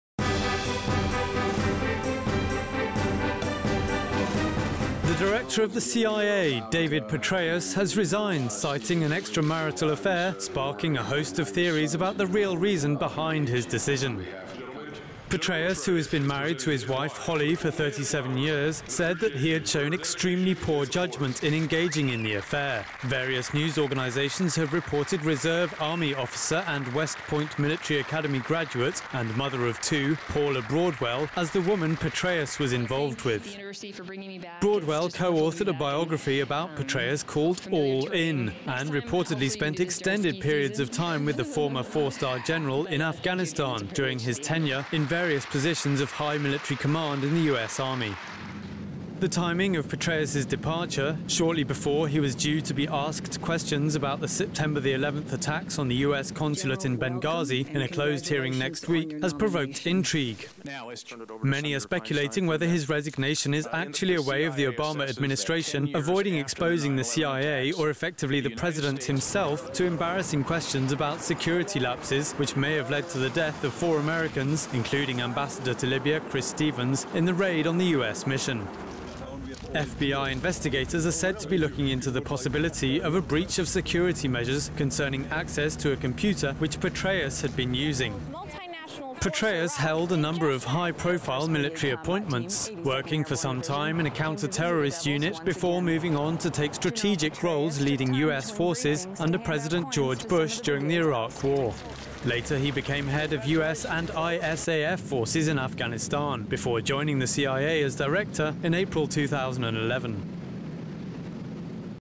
Petraeus resigns citing extra-marital affair
The director of the CIA, David Petraeus, has resigned citing extramarital affair, sparking a host of theories about the real reason behind his decision.

Petraeus, who has been married to his wife Holly for 37 years, said that he had shown extremely poor judgement in engaging in the affair; various news organisations have reported reserve army officer and West Point Military Academy graduate and mother-of-two Paula Broadwell as the woman Petraeus was involved with.

Broadwell co-authored a biography about Petraeus called "All In", and reportedly spent extended periods of time with the former four star general in Afghanistan during his tenure in various positions of high military command in the US army.

The timing of Petraeus's departure, shortly before he was due to be asked questions about the September 11 attacks on the US consulate in Benghazi in a closed hearing next week, has provoked intrigue.

Many are speculating whether his resignation is actually a way of the Obama administration avoiding exposing the CIA or effectively the President himself to embarrassing questions about security lapses which may have led to the death of four Americans including Ambassador to Libya Chris Stevens in the raid on the US mission.

FBI investigators are said to be looking into the possibility of a breach of security measures concerning access to a computer which Petraeus had been using.

Petraeus held a number of high-profile  military appointments, working for some time in  a counter-terrorist unit before moving on to take strategic roles leading US forces under President George Bush during the Iraq War. Later he became head of US and ISAF forces in Afghanistan before joining the CIA as director in April 2011.